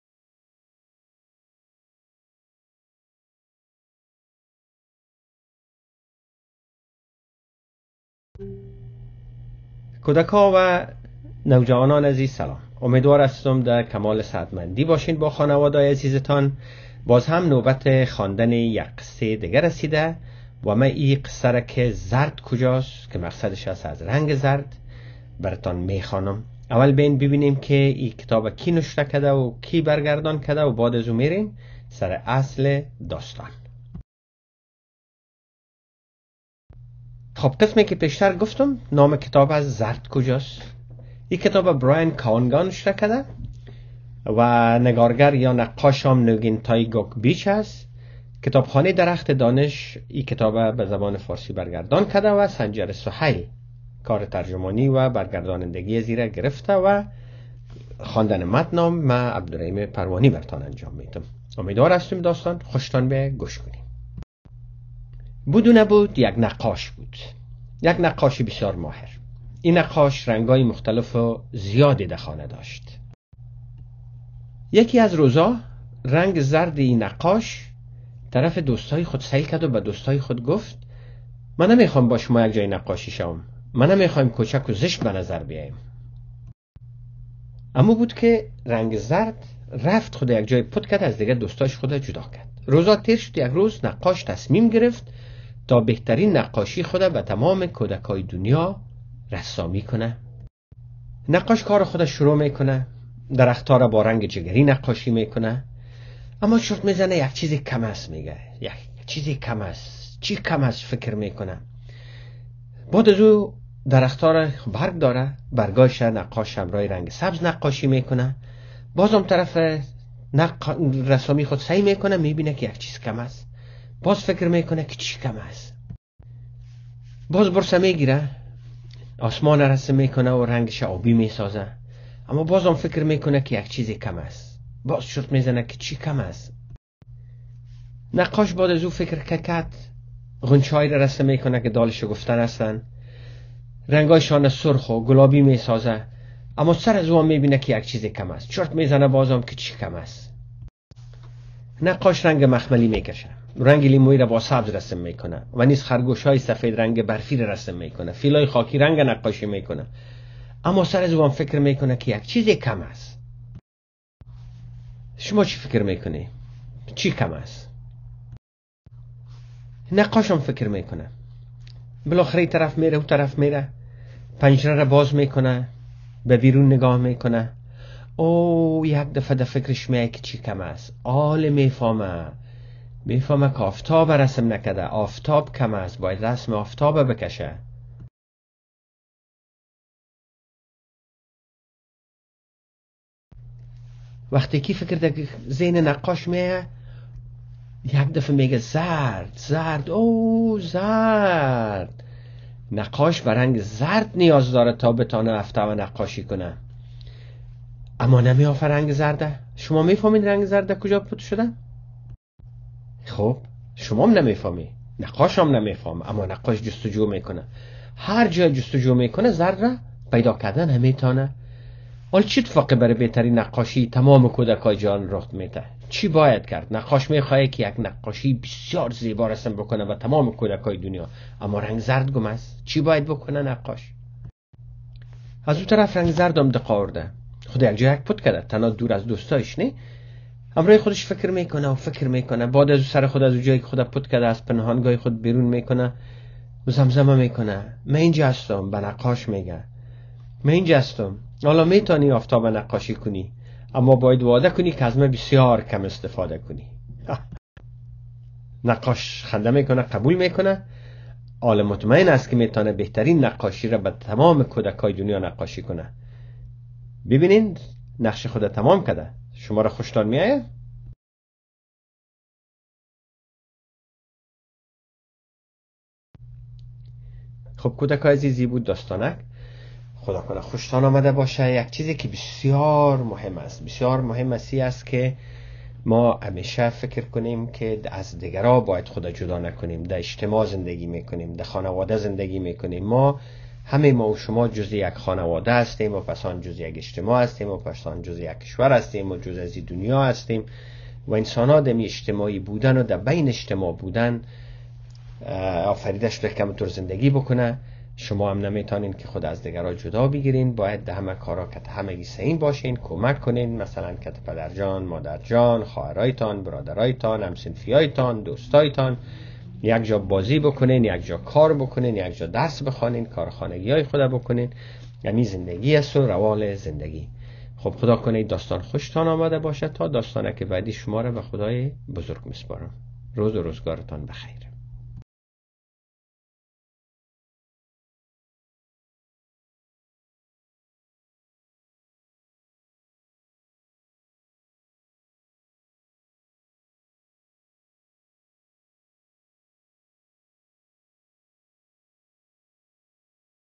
رنگ زرد کجاست؟
نقاشی، تصویر بسیار زیبایی برای تمام کودکان دنیا رسیم کرده است. ھمه‌ی‌ رنگ‌ھای دنیا دوست دارند تا بخشی از این تصویر باشند، اما زرد نمی‌خواھد. او تصمیم می‌گیرد فرار کند و تصویر را ناتمام بگذارد. رسام چی باید بکند؟‌ آیا می‌تواند بدون رنگ زرد نقاشی کند؟

برای به مطالعه کتاب‌ها و مواد آموزشی به سایت کتاب‌خانه درخت دانش بروید: